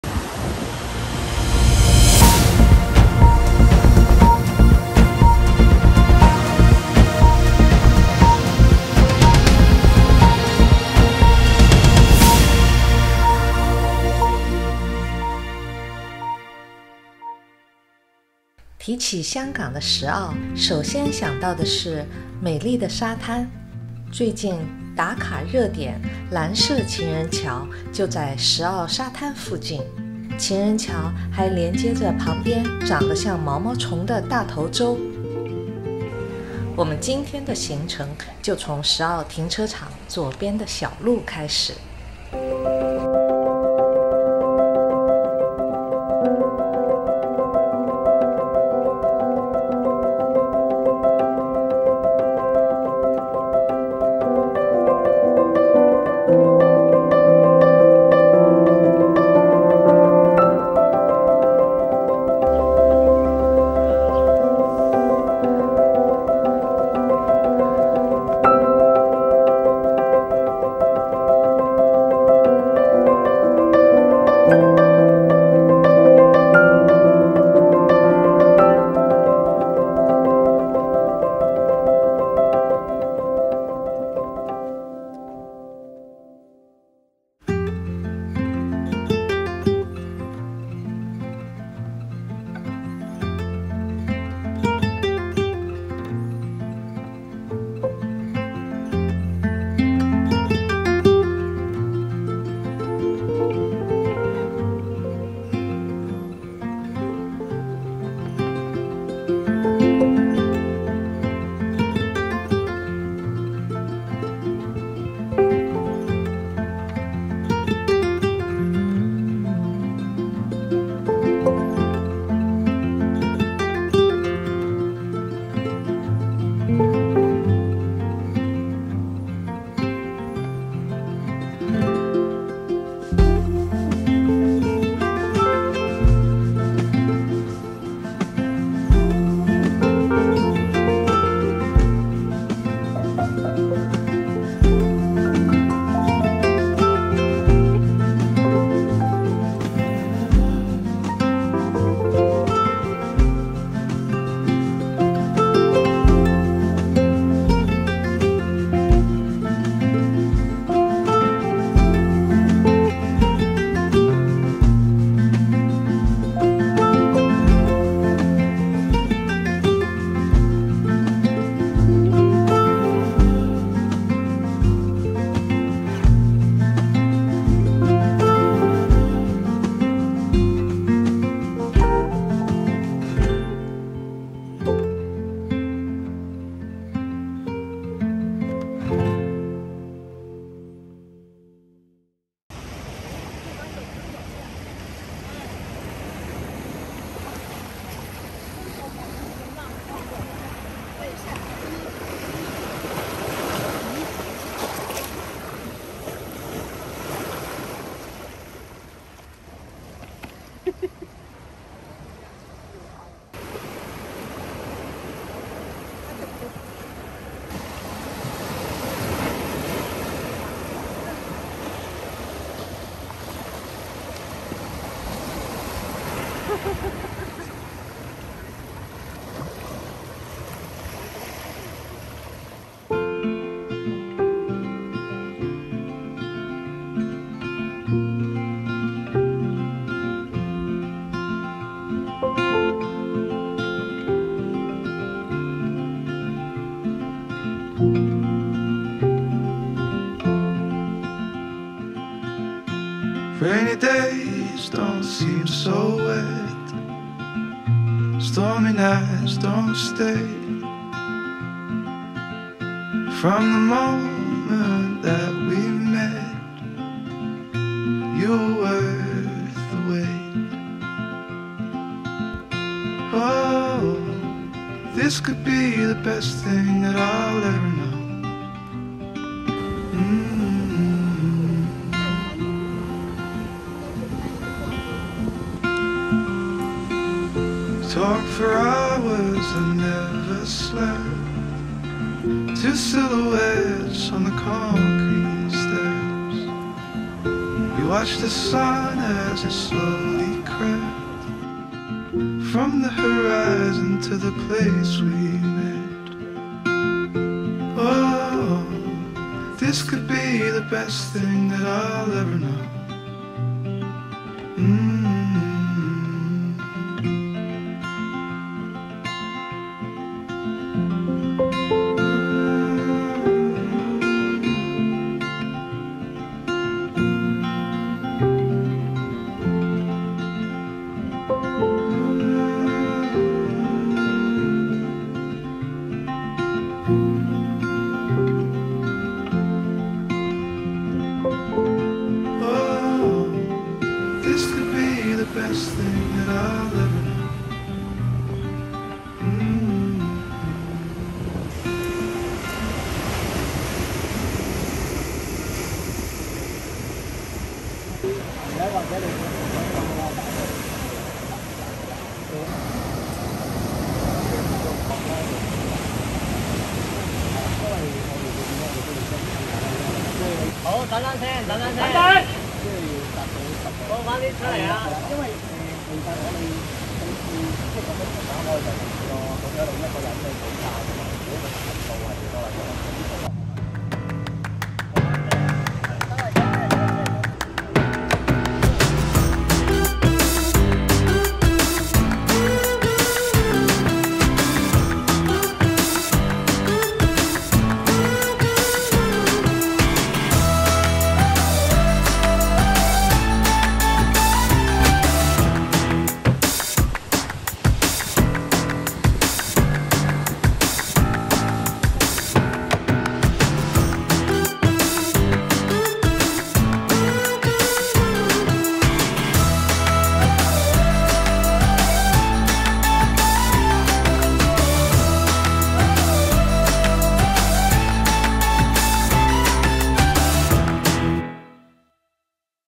【香港好去处】石澳｜大头洲｜轻松郊游路线｜Shek O ｜情人桥｜蓝桥｜航拍 ｜Lovers Bridge ｜Tai Tau Chau
石澳 Shek O 情人桥 大头洲 航拍 行山路线 好去处 香港 Hong Kong Travel#Hiking#Lovers Bridge#Tai Tau Chau# iphone 12#Dji air 2

石澳以广阔的泳滩而著名，但原来沿石澳山仔仔路，行到路的尾端，还可以欣赏广阔的香岛湾，在这里进入楼梯而下，可以到达连接到大头洲的蓝色「情人桥」。这条蓝色小桥，桥身只够两人同时通过，加上浪漫的蓝色而得名，因此有太多新人都被这里的景色吸引，来拍摄婚纱照。
大头洲受到海浪侵蚀的情况亦比较强烈，又留下很多海浪侵蚀的痕迹，因此造就了很多奇石及攀石的好地方。这里的路亦相当易走，除了可欣赏海蚀痕迹，还能轻轻松松欣赏到海天一色的景致，是一个不错的郊游好去处。